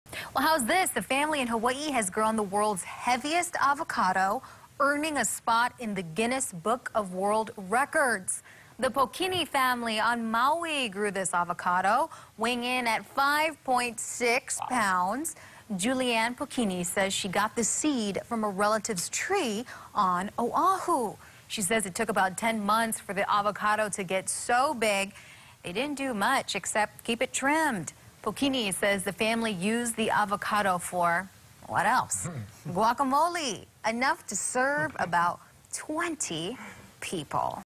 Maui family makes it in Guinness Book of World Records
...for growing a heavy avocado.